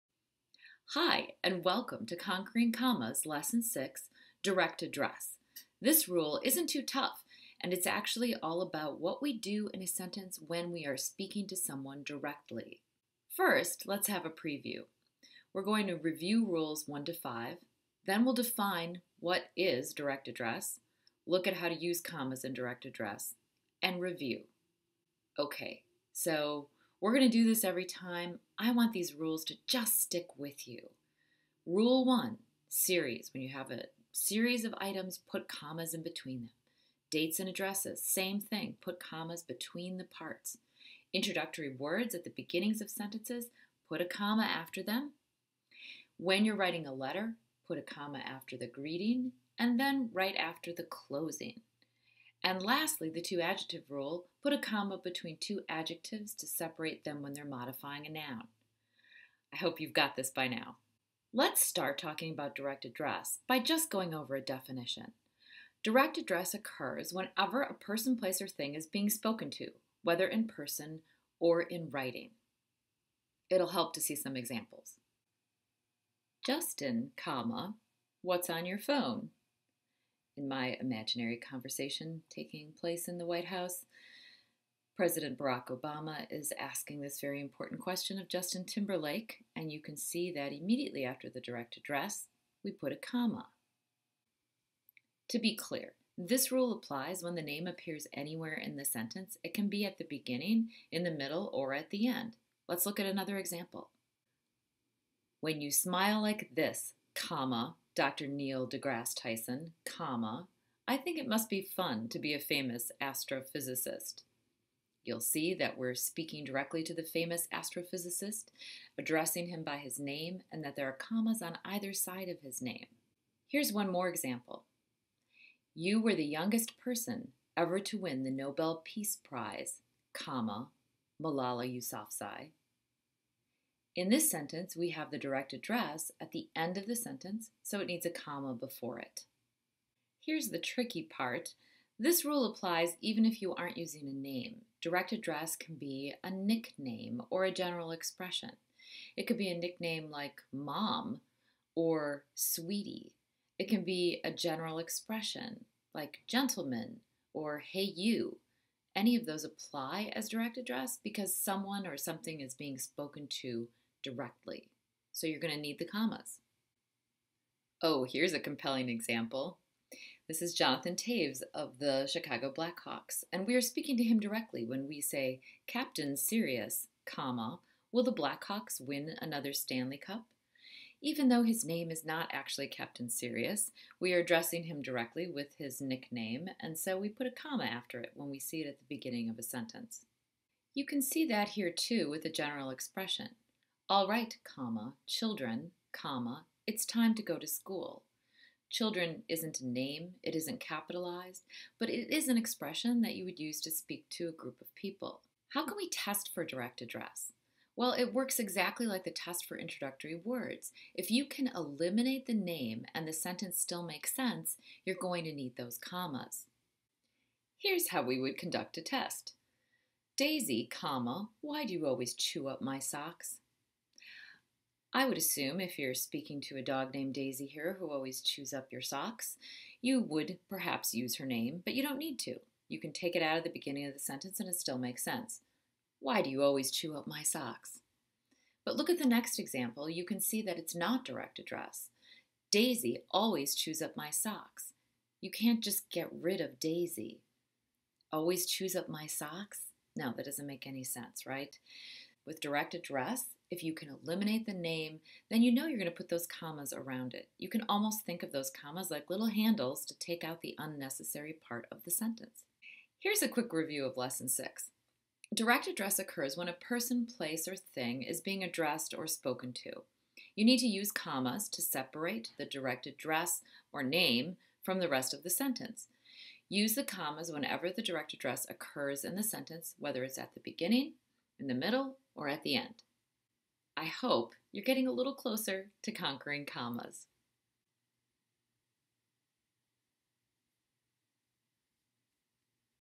Conquering Commas Lesson 6 - Direct Address
In this lesson, you'll learn all about how to use commas in direct address.